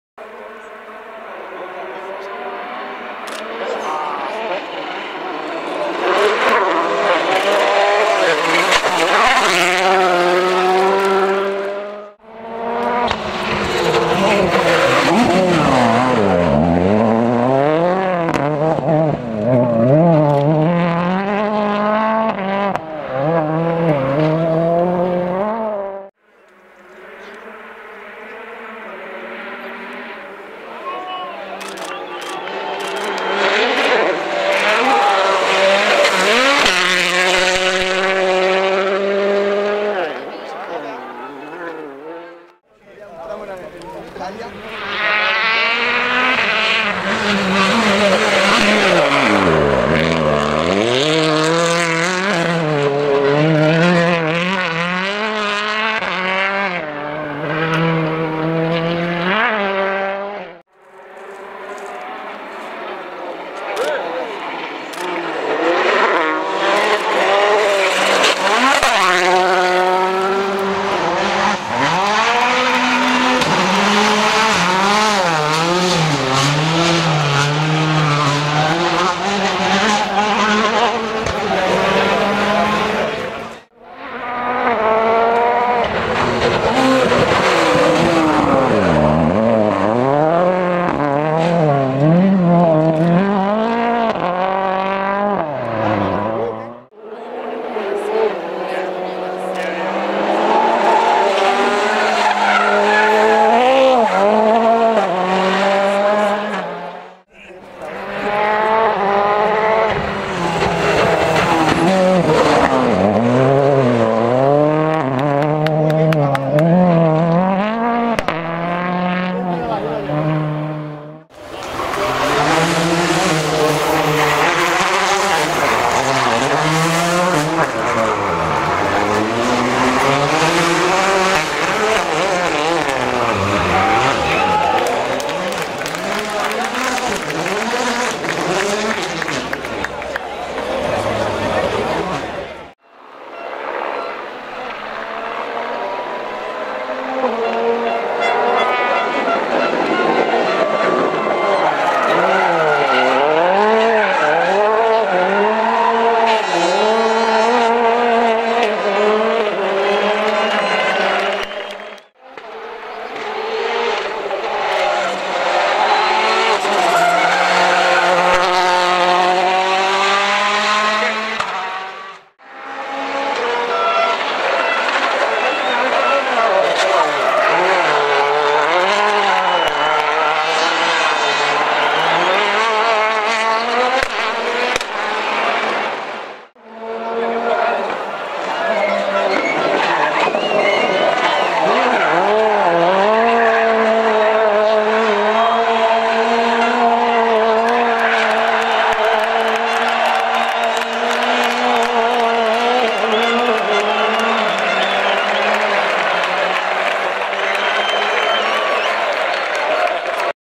WRC Rally Catalunya 2014 (HD)
In this video we have
0:05 OGIER Sébastien INGRASSIA Julien FRA/FRA VOLKSWAGEN Polo R WRC

0:14 HIRVONEN Mikko LEHTINEN Jarmo FIN/FIN FORD Fiesta RS WRC
0:30 SORDO Daniel MARTI Marc ESP/ESP HYUNDAI i20 WRC
0:45 PADDON Hayden KENNARD John NZL/NZL HYUNDAI i20 WRC
1:00 MIKKELSEN Andreas FLOENE Ola NOR/NOR VOLKSWAGEN Polo R WRC
1:11  AL QASSIMI Khalid PATTERSON Chris ARE/IRL CITROËN DS3 WRC
1:24 KUBICA Robert SZCZEPANIAK Maciej POL/POL FORD Fiesta RS WRC
1:48 PROTASOV Yuriy CHEREPIN Pavlo UKR/UKR FORD Fiesta RS WRC
2:23  EVANS Elfyn BARRITT Daniel GBR/GBR FORD Fiesta RS WRC
2:43  OSTBERG Mads ANDERSSON Jonas NOR/SWE CITROËN DS3 WRC
2:52 MEEKE Kris NAGLE Paul GBR/IRL CITROËN DS3 WRC
 3:37 BLOCK Ken GELSOMINO Alex USA/ITA FORD Fiesta RS WRC

Clips are from Shakedown
SS 4 Terra Altra 1 & 2
SS11 Escaladei
SS15/17 Riudecanyes @ Coll de Teixeta roundabout!!